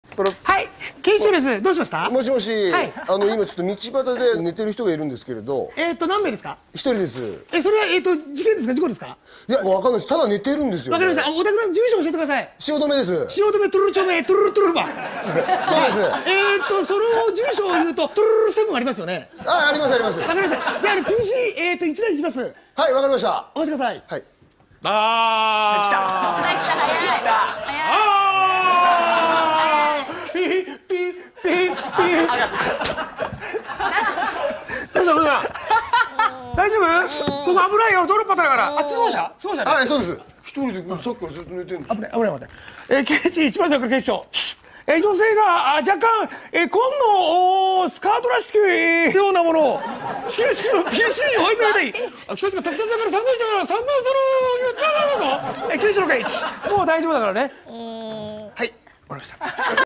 【柳沢慎吾】爆笑する警察コント、これはもう芸術だよｗｗｗ
慎吾ちゃんの警察コント、きましたｗ　芸が本当に、こまかくて面白いですよねｗ